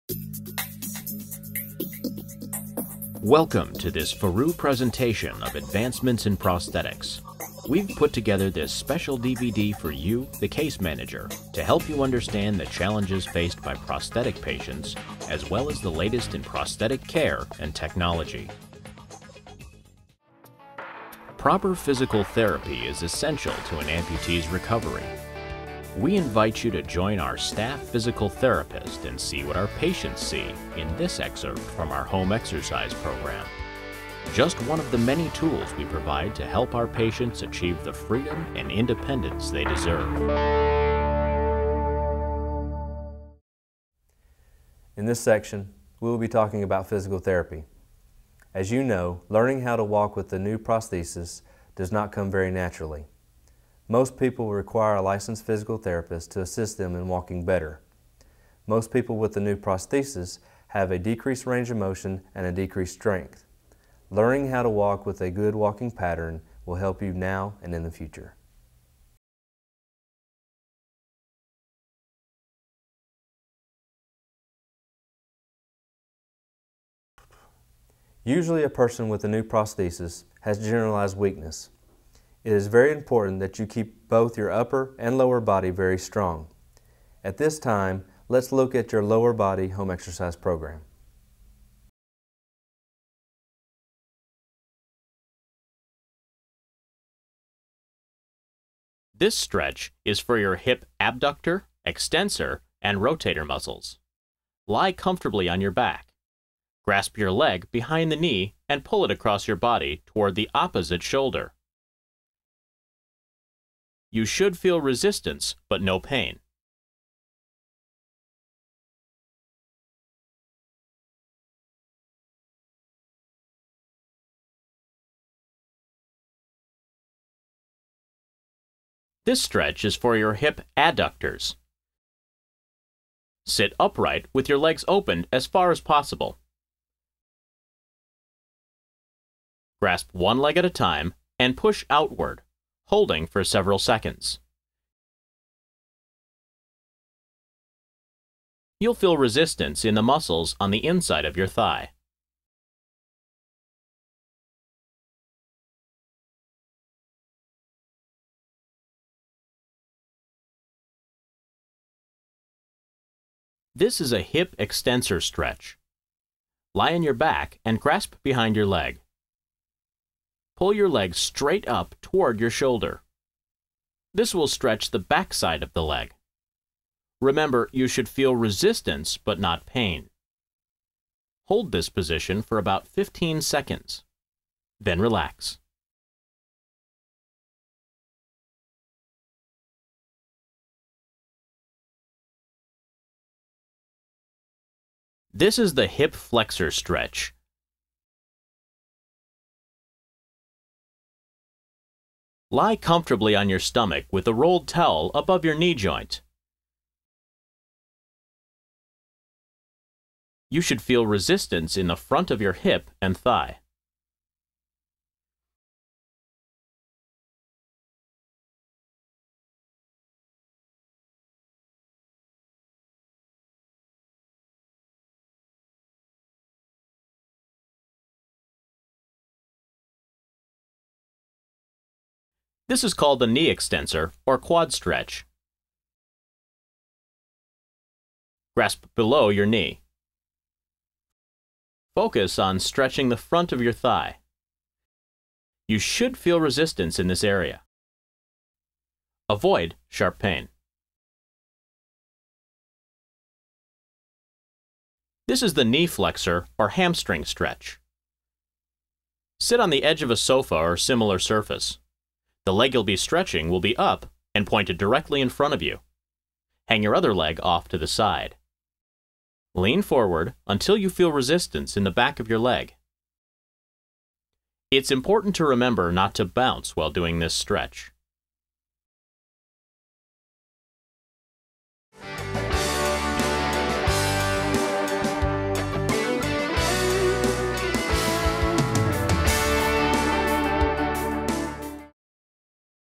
Advancements in Prosthetics - Prosthetics Physical Therapy Program - Part 01
Welcome to this Fourroux presentation of Advancements in Prosthetics.  We've put together this special DVD for you, the Case Manager, to help you understand the challenges faced by prosthetic patients, as well as the latest in prosthetic care and technology.

The Journey

We begin with "The Journey," an insightful program that includes hopeful one on one interviews and a unique window into patients lives from amputation, through rehabilitation and to recovery.  "The Journey" is a great tool for helping new amputees.

Inside Your Socket

Next, the socket has been described as the most important part of a prosthesis.  Learn the basics of lower extremity socket design, fitting, liners, socks, and more in our presentation entitled "Inside the Socket."

Prosthetic Physical Therapy Program

Finally, proper physical therapy is essential to an amputee's recovery.  We invite you to join our staff Physical Therapist and see what our patients see in this excerpt from our home exercise program.  Just one of the many tools we provide to help our patients achieve the freedom and independence they deserve.